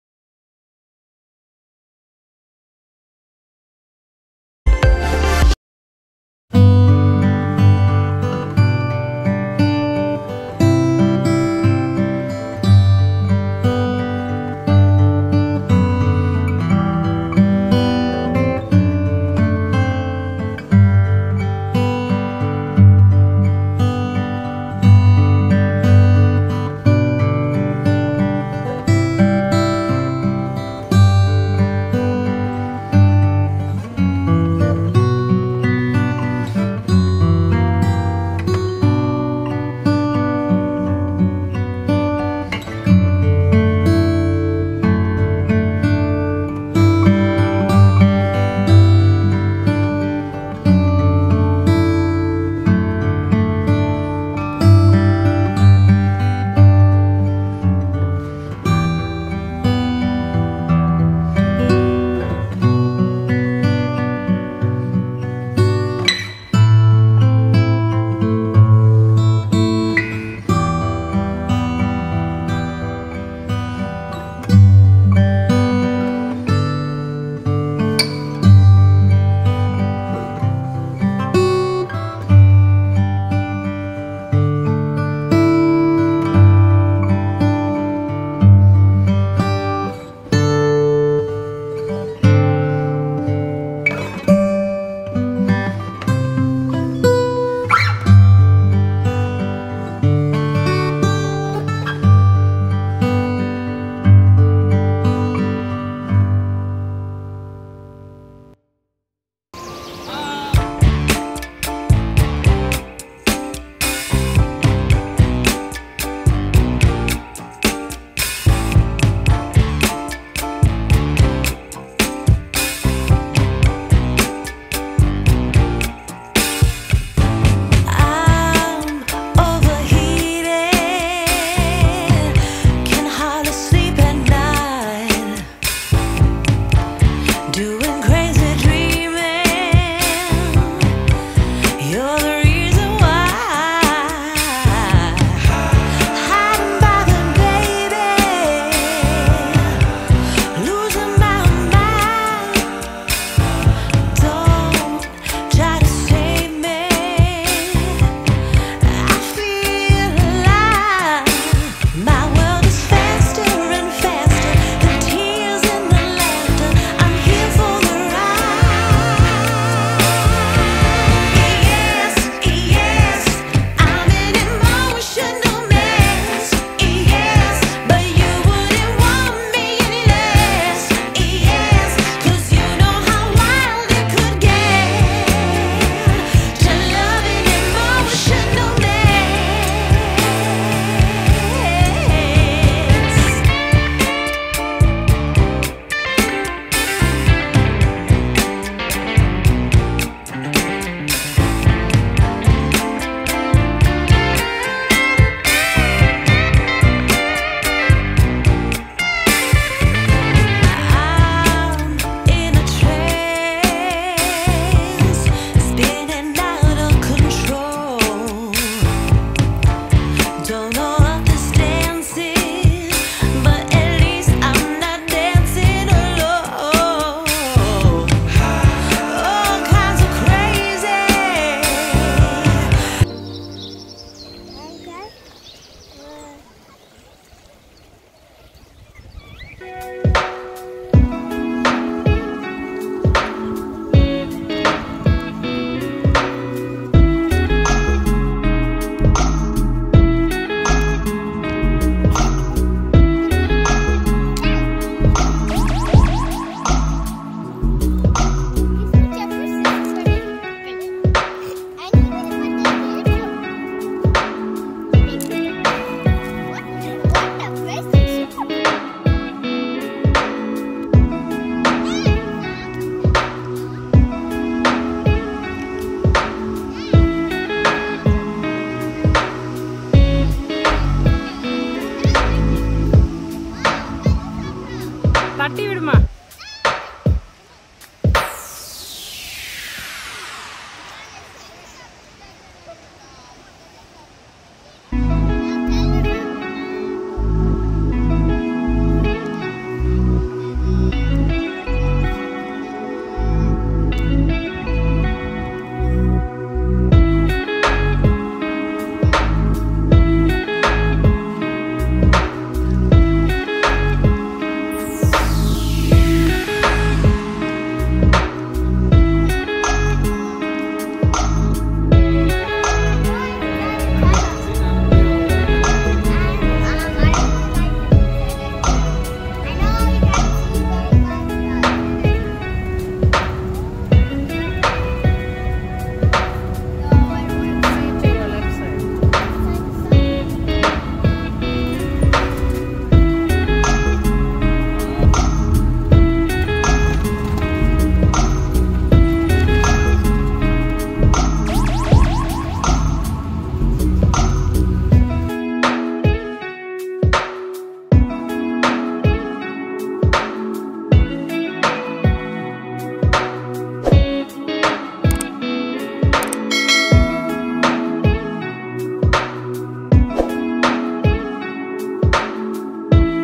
Visit Yosemite Without Reservation
Shorts Visit yosemite national park with no reservation.
When yosemite reopened after 2020 pandamic yosemite visit needs reservation. Starting May 2021 they allow only people have prior reservation. for more details.

in this video I am going to share how you can visit yosemite without reservation.